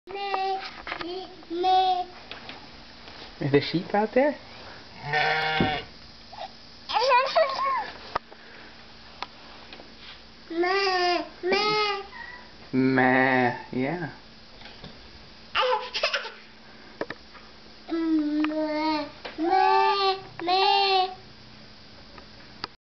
Hazel & sheep 8-15-11
A higher res version than the one found on my blog. H talks to sheep, sheep talk back.